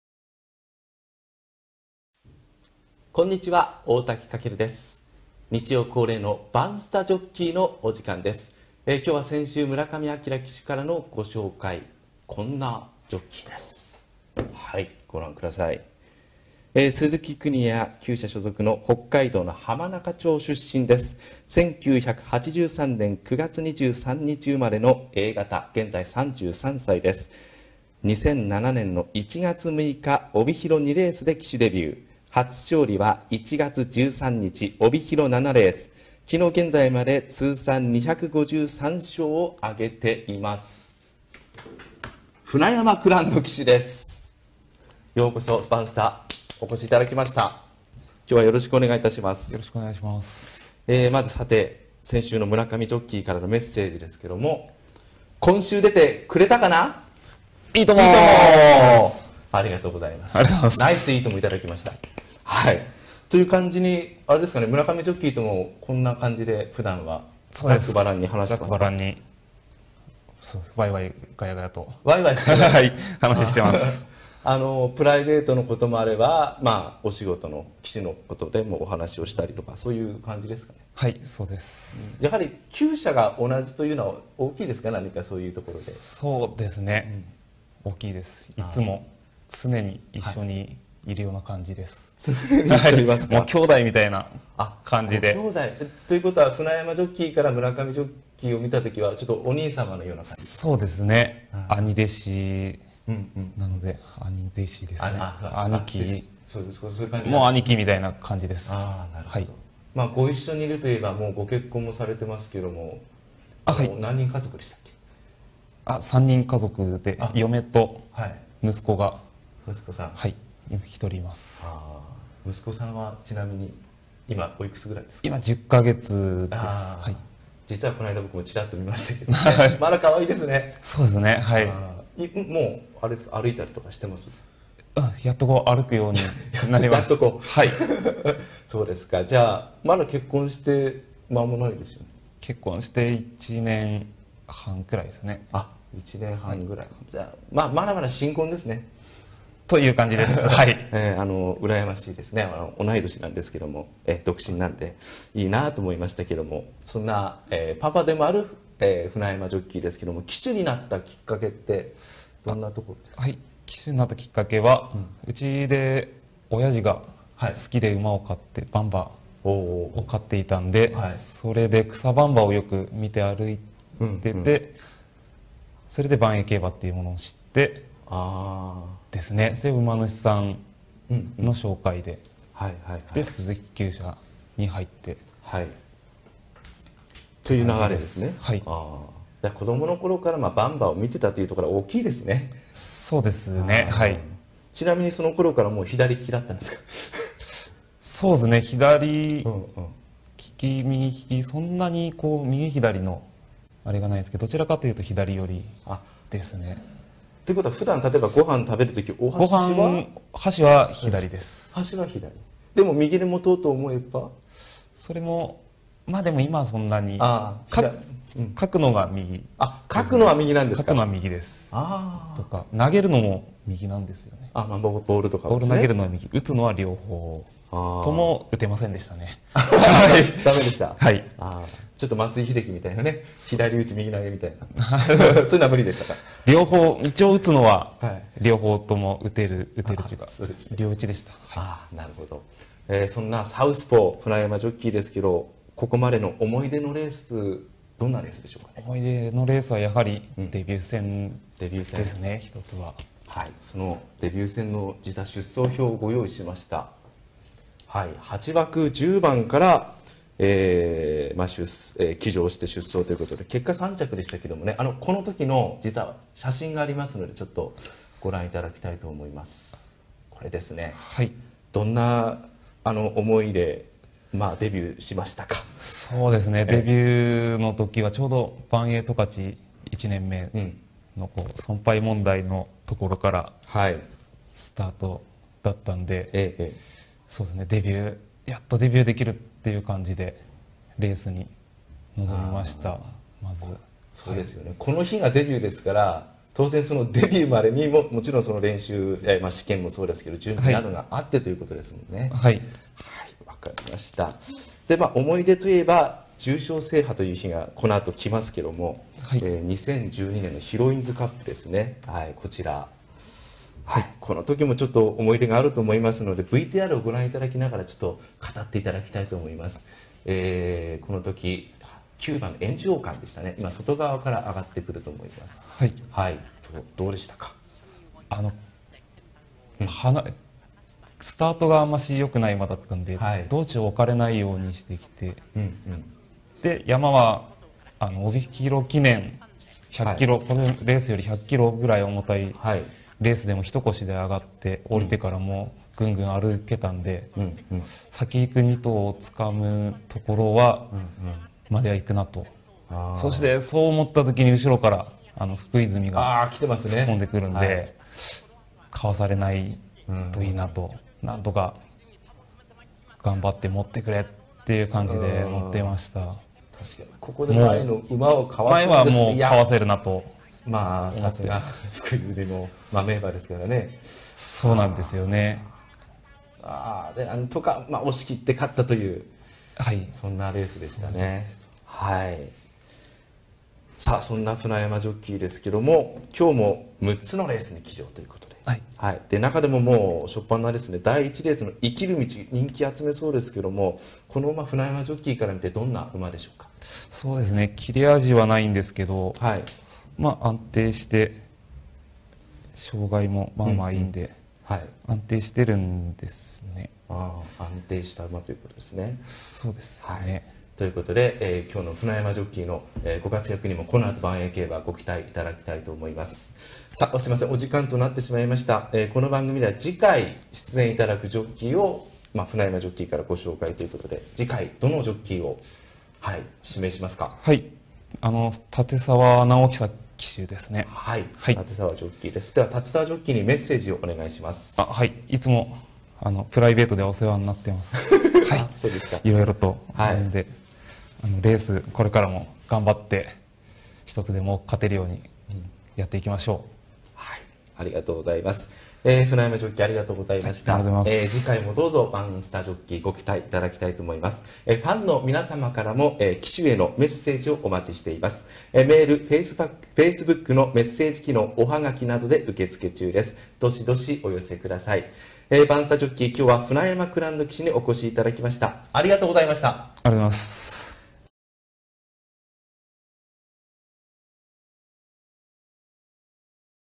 第７回ばんスタジョッキー 船山蔵人騎手 (2016.6.26)
日曜恒例ばんスタジョッキー。先週の村上章騎手からのご紹介、船山蔵人騎手に登場して頂きました。サウスポの船山騎手のお話し必見です。
（プロフィールの鈴木調教師の名前が違っておりました。正　鈴木邦哉調教師　です。大変失礼いたしました。お詫び申し上げます。）